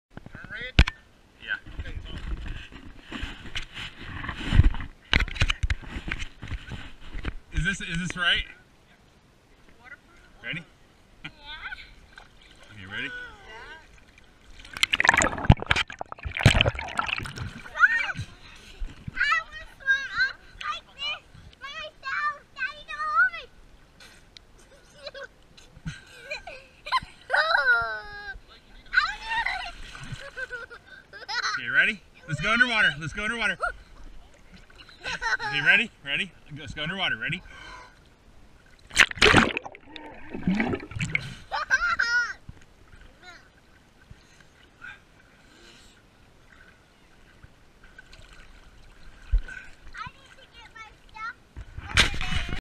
Blake and Cecilia
Dad giving his daughter a few lesson in pool